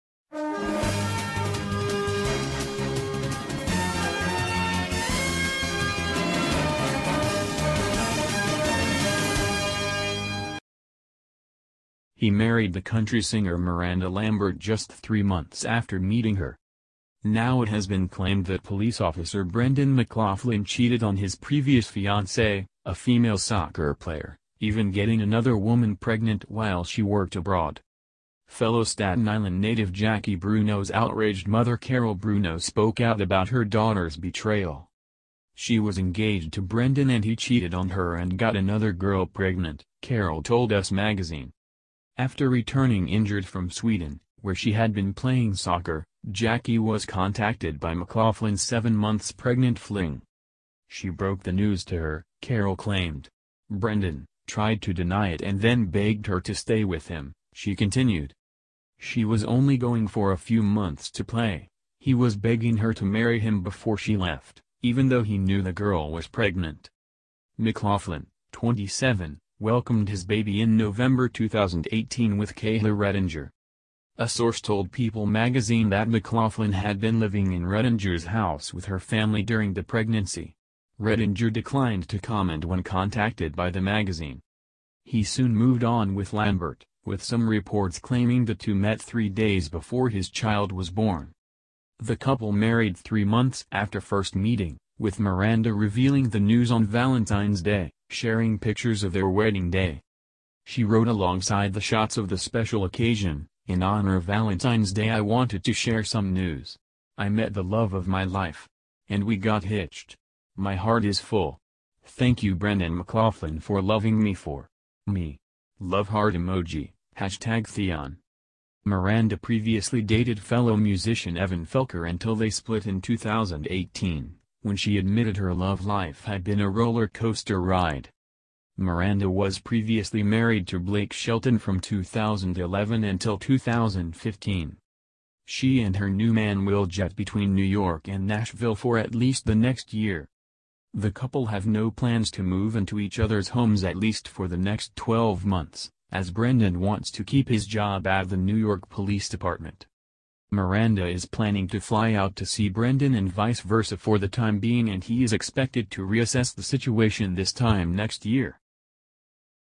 Miranda Lambert's new husband 'cheated on soccer player fiance and got another woman pregnant'
Miranda Lambert's new husband 'cheated on soccer player fiance and got another woman pregnant'... before marrying singer weeks after they met